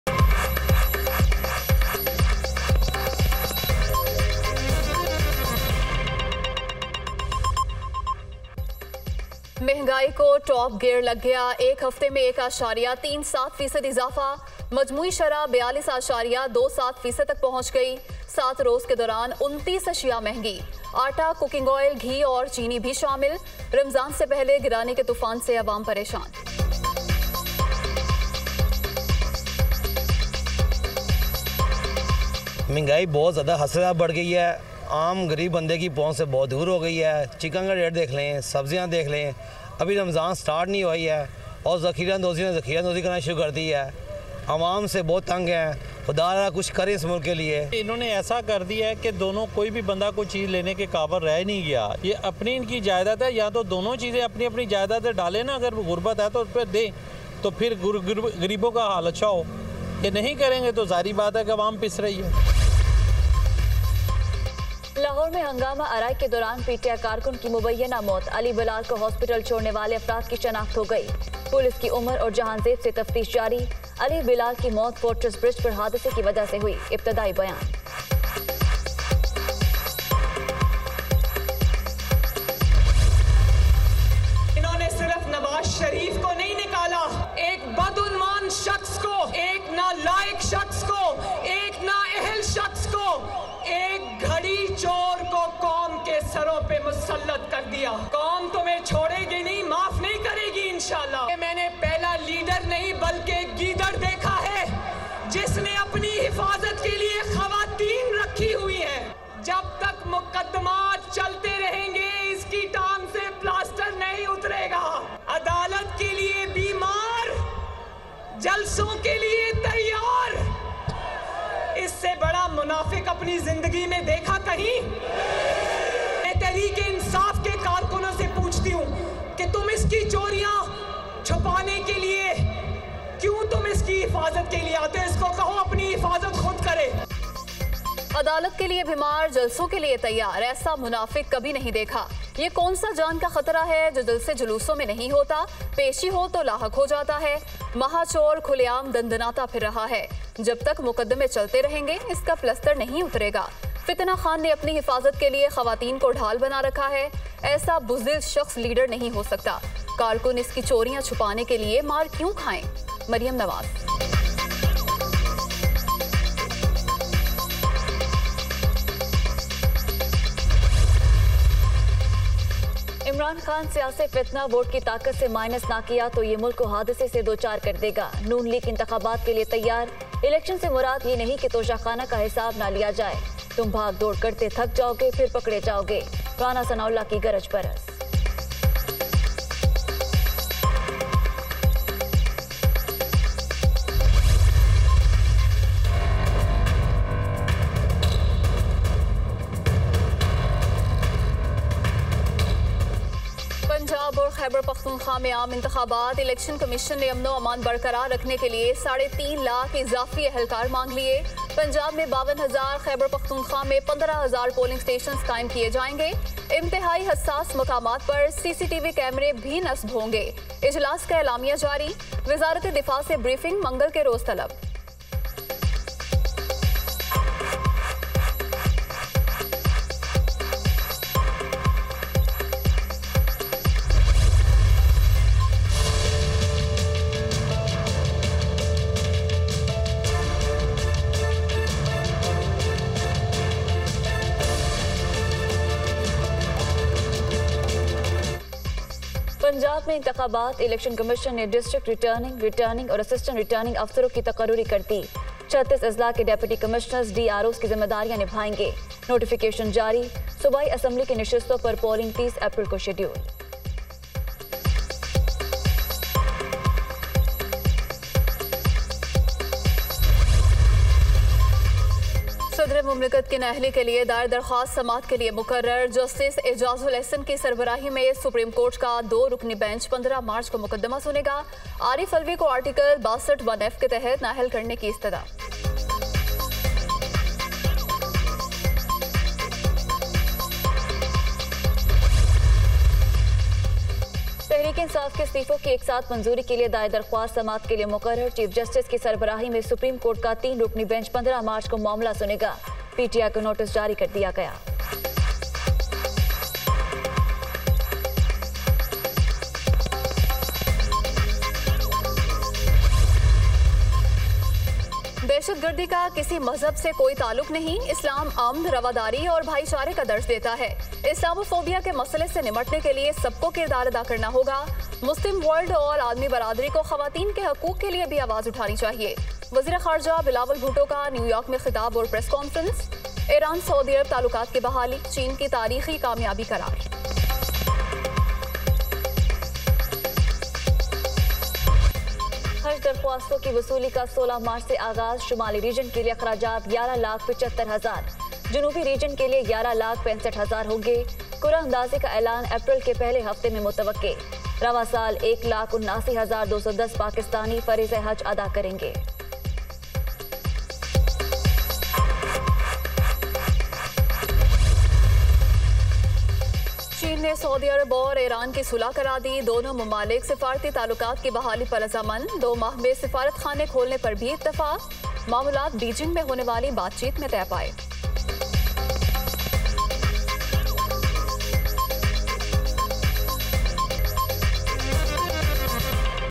Supreme Court In Action | Dunya News Headlines 10:00 AM | 11 March 2023
Maryam accuses Imran of economic meltdown

Imran vows to move against Punjab administration over Lahore incident

Imran Khan heaves a sigh of relief as BHC suspends arrest warrant

PSL 8: Rossouw, Pollard hold key as Sultans chase record victory against Zalmi

Punjab election: ECP appoints returning officers

Gilani says PPP to contest elections on its own symbol